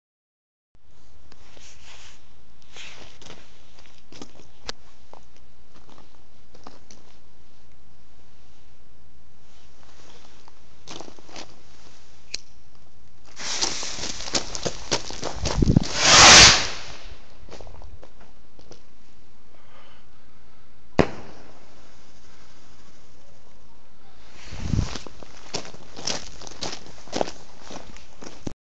Rocket with Shimizu Red Twinkler Stars
4 oz.Nozzleless BP Rocket with 2" Festival Ball Shell Header
Fireworks: The Art, Science, and Technique Author: Dr. Takeo Shimizu Page: 224